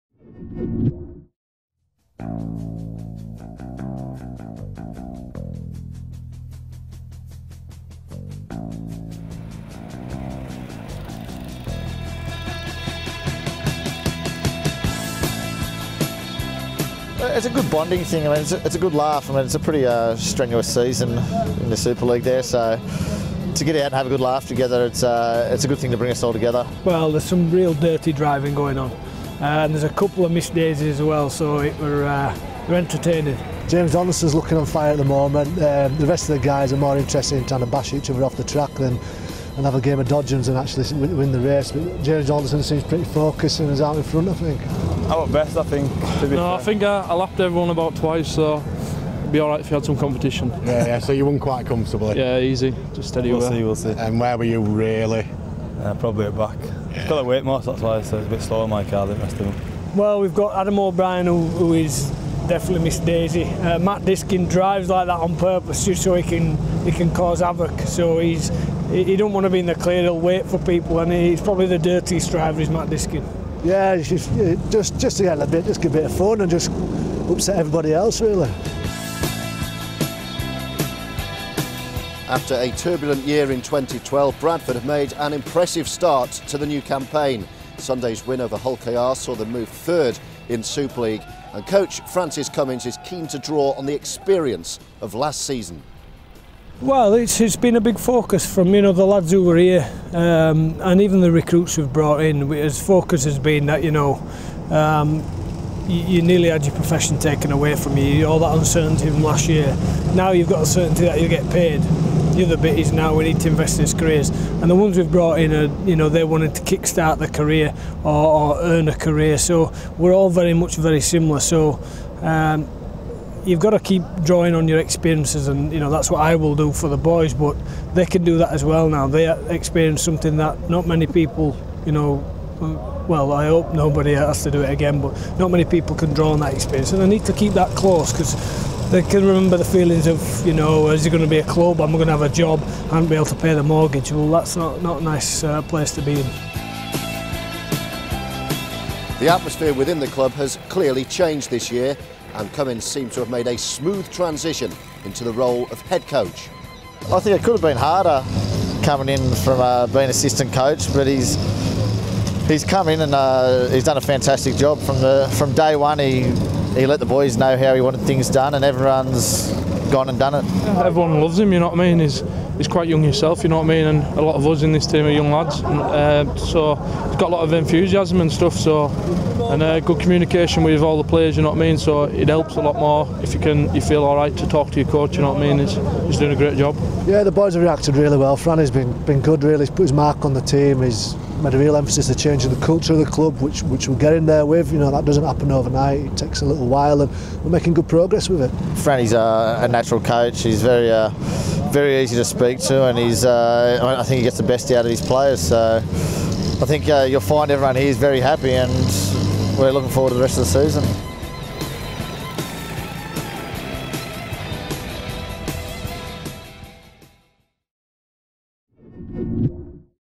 Cummins hits top gear
Bradford head coach Francis Cummins has made a smooth transition into the role this season following his promotion from assistant.
The Bulls are sitting third in Super League and Cummins is proving to be a big hit with the squad. SLTV went along to a team go karting session to get the inside track...